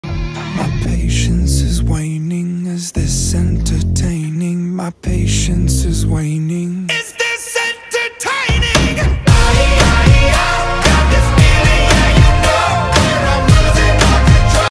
BodyGuard Vs Dog 🐶 @ Sulcata Tortoise 🐢
We bathing our Sulcata on Sunday , Male Sulcata acting protect 2 femal Sulcata from dog.